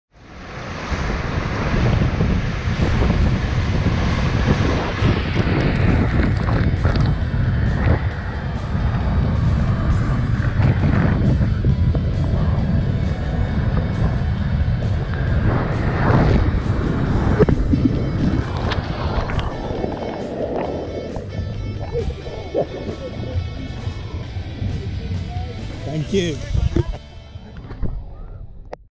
DAREDEVIL’S PEAK, THE TALLEST WATERSLIDE IN NORTH AMERICA, PERFECT DAY AT COCOCAY - 4K
The 135-foot-tall Daredevil’s Peak at Perfect Day at CocoCay is the tallest waterslide in North America.
4K POV footage of Daredevil's Peak at Perfect Day at CocoCay.
Perfect Day at CocoCay, Royal Caribbean.